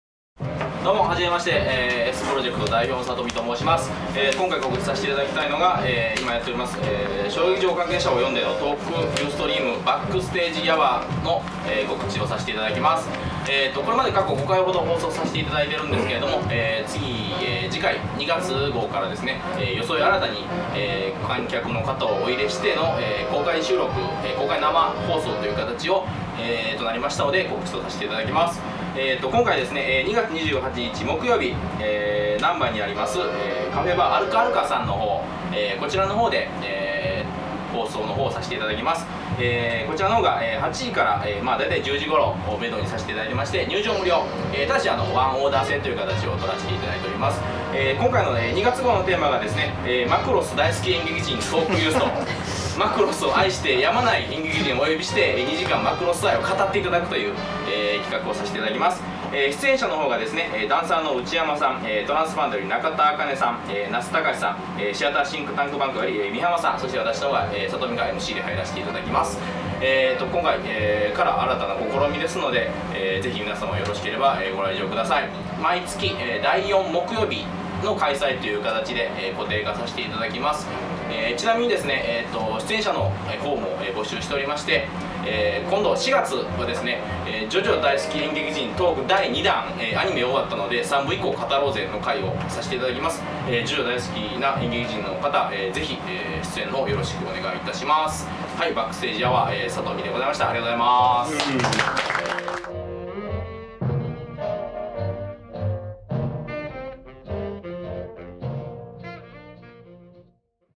【SPCM】BACK STAGE 夜話～マクロスだいすき～ 告知
★日時：2013年2月28日(木) 20：00～
★会場：大阪・なんば あるかアるか
 　　　  近鉄日本橋駅から徒歩4分
★料金：入場無料（ワンオーダー制）